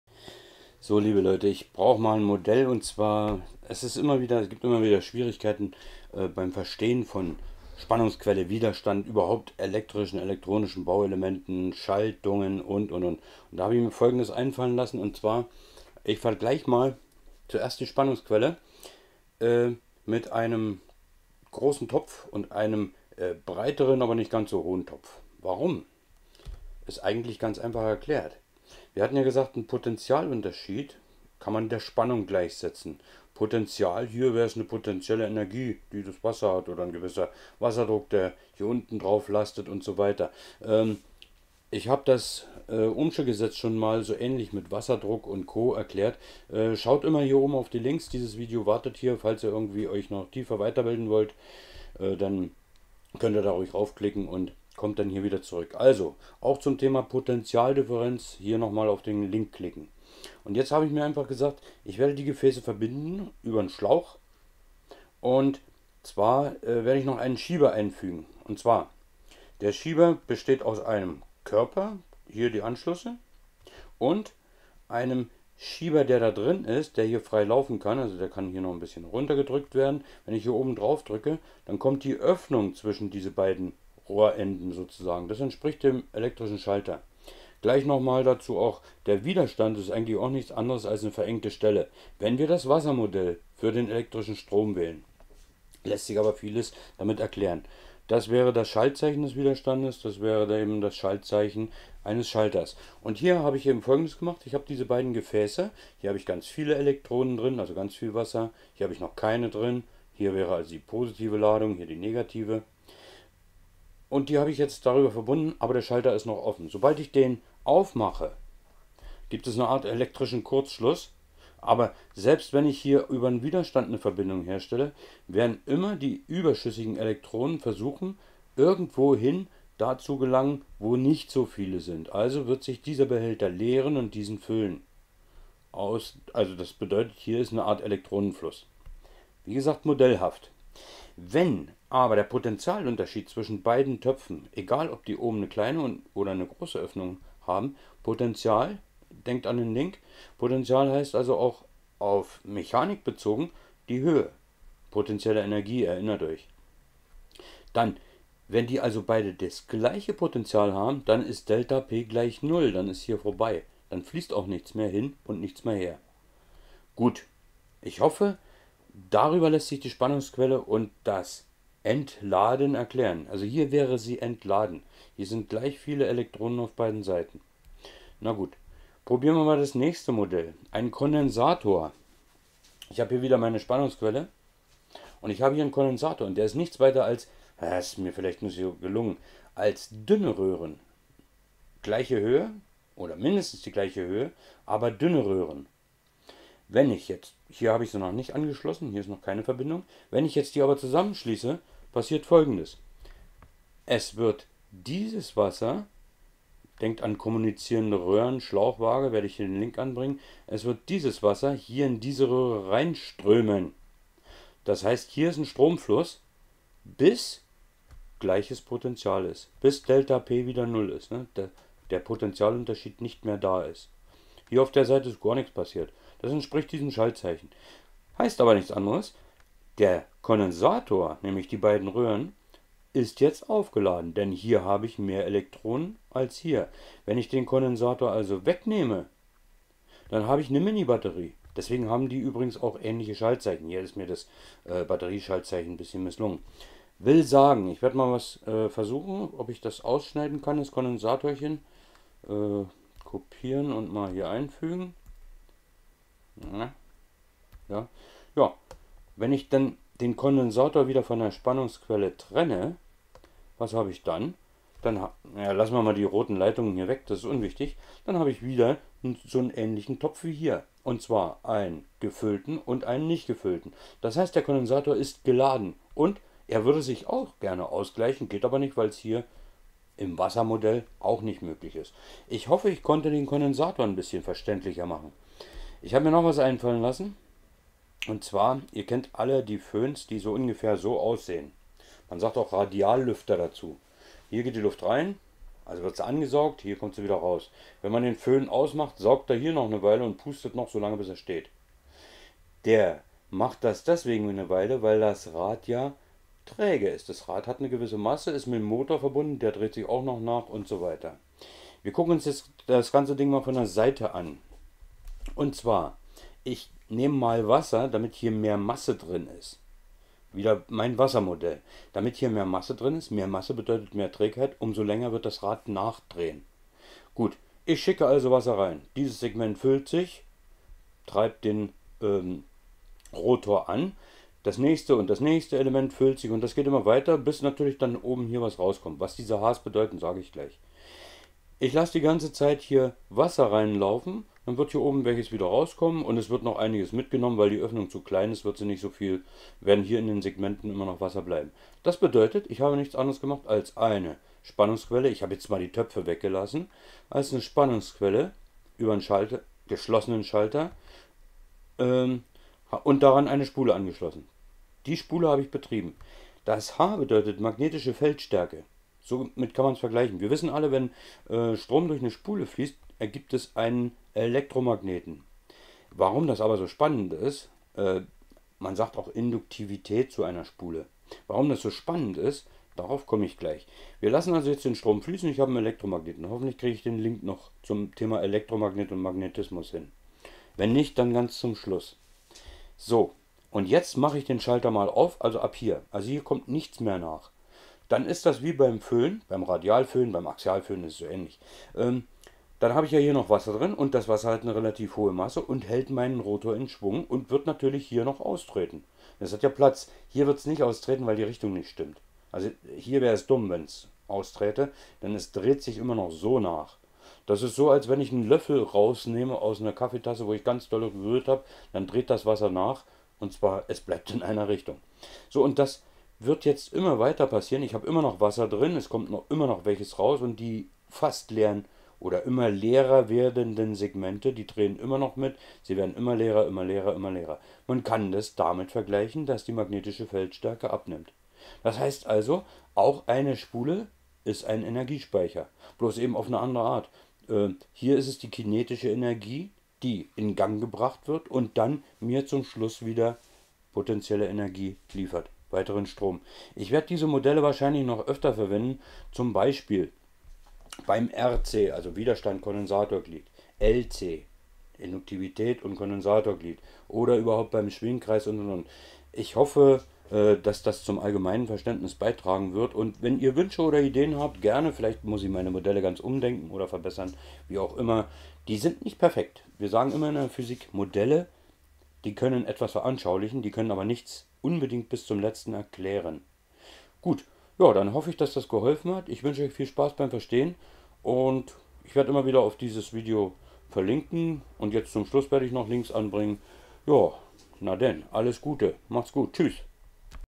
Einfach: Modell für Spule, Kondensator, Widerstand, Schalter, Spannungsquelle als Erklärungs-Hilfe
Bitte, helfen Sie uns Endlich wieder (Sapere aude!) Meine Modelle sollen das Verstehen von Induktivität, Kapazität, Widerstand, Spannungsquelle, Potential(-Ausgleich), Potentiometer, Schwingkreis, RC-Glied, LC-Glied usw. erleichtern helfen.

Björn Köhler

zzzzz_intern